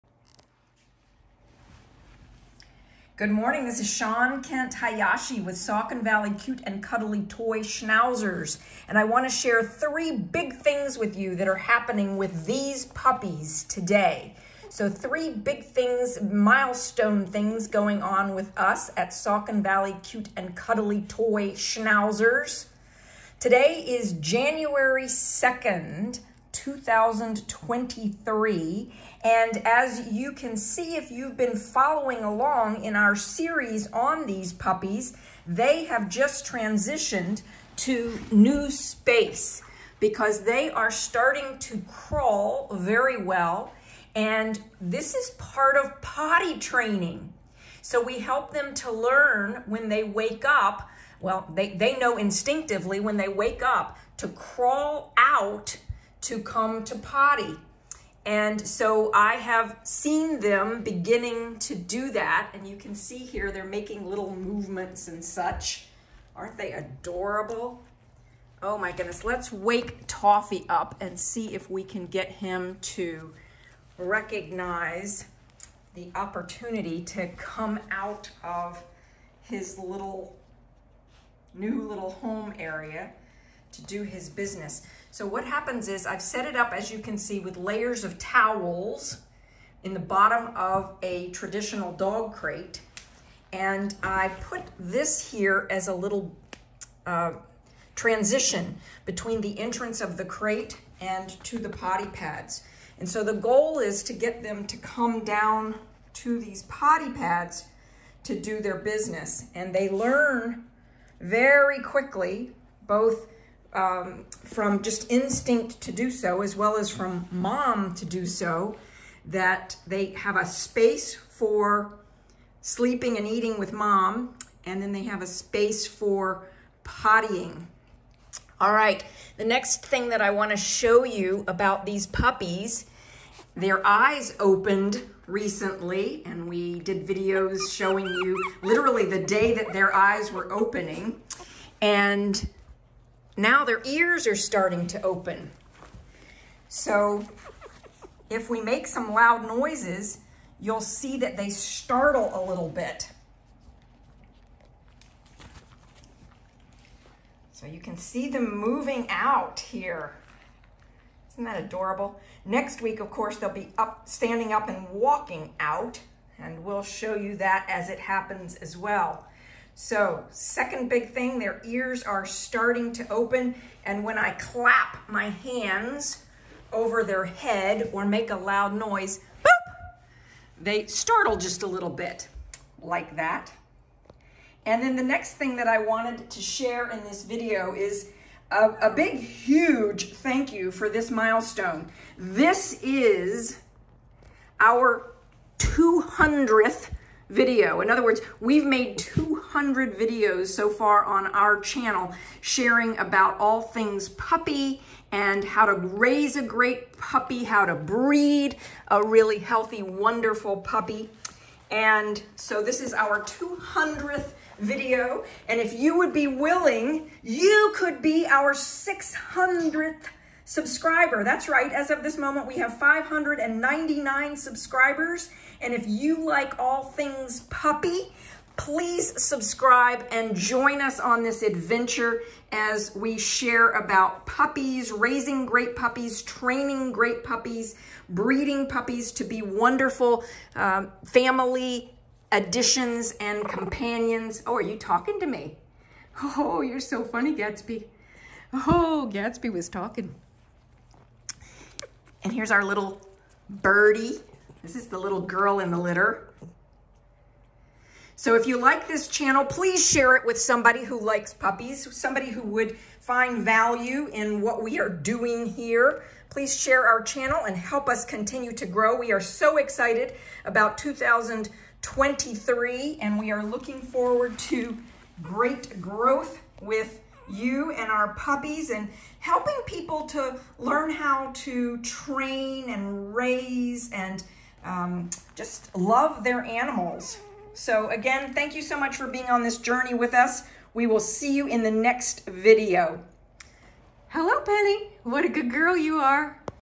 SVCC SCHNAUZER Puppy Nursery - 3 BIG MILESTONES!!
Sharing 3 BIG Milestones in the SVCC TOY SCHNAUZER Puppy Nursery
We love our Mini and Toy Schnauzer PUPPIES!
Shawn Kent Hayashi shows you how to set your puppy up for success and train your puppy to be a great family pet.